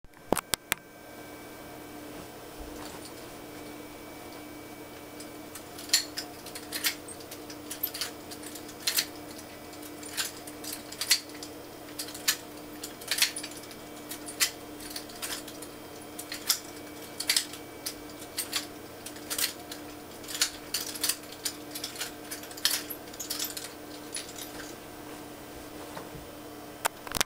F2B & B2F Wrist Spins in Reverse Fig8 [Freechaku] ~ SSFNA ~ WDS
Integrating wrist spins into the reverse figure eight pattern.  ~